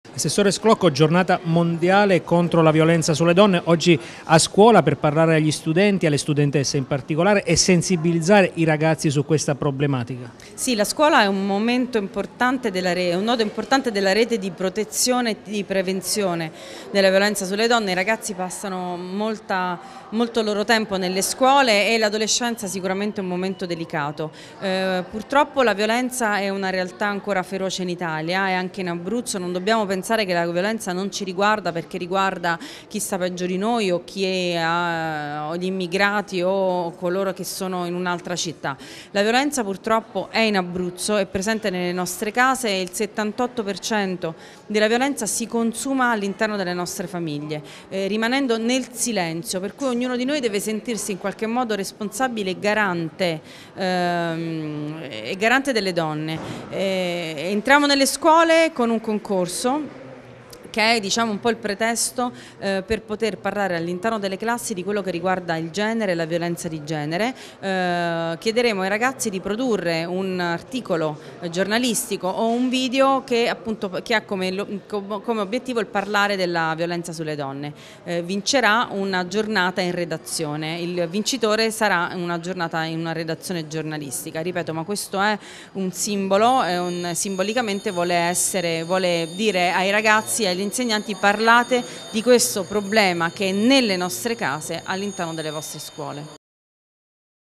Sclocco per la giornata mondiale sulla violenza donne
Nella giornata mondiale contro la violenza sulle donne, oggi a Pescara, presso l'Istituto Marconi si è tenuto un incontro a cui ha partecipato l'assessore regionale alle Pari Opportunità Marinella Sclocco